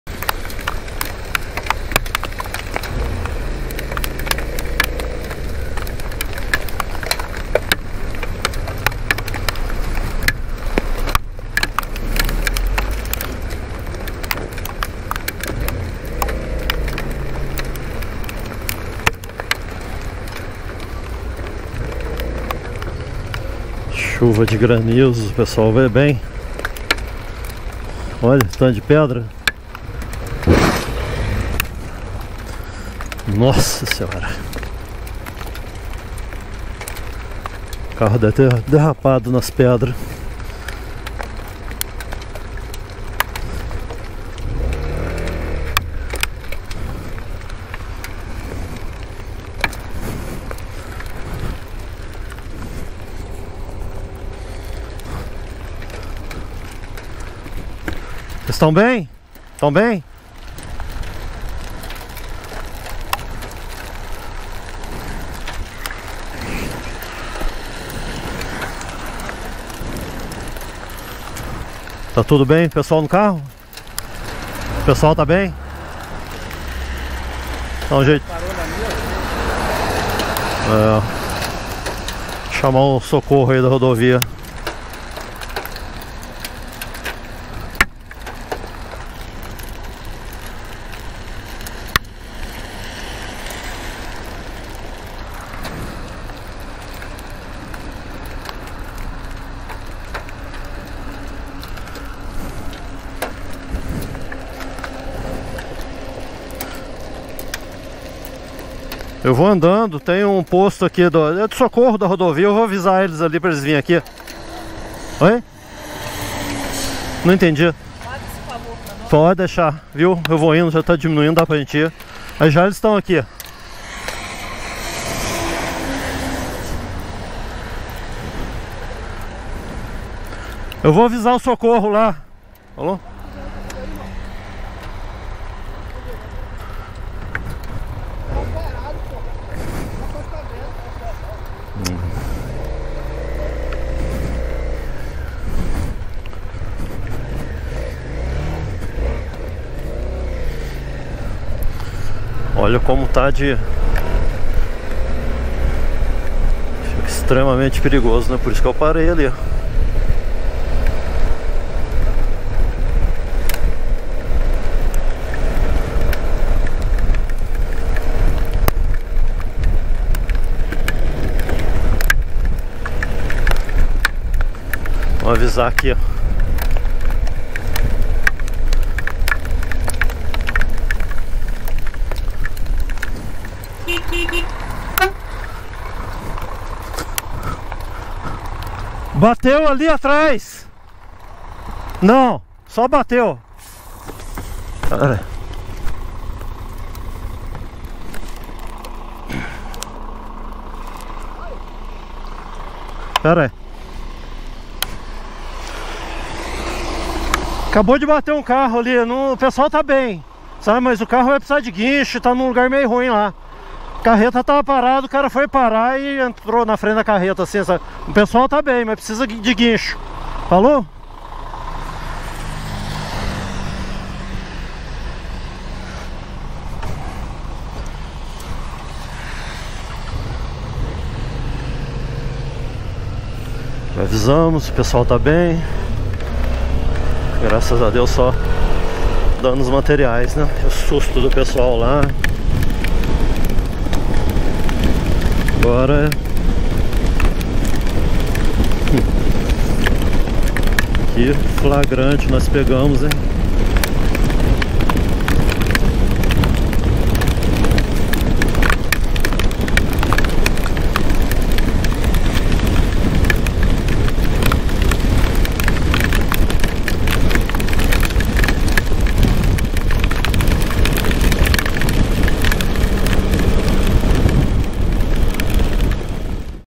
FILMEI UM ACIDENTE!
Um grande susto no retorno da última viagem, mas graças a Deus apenas danos materiais!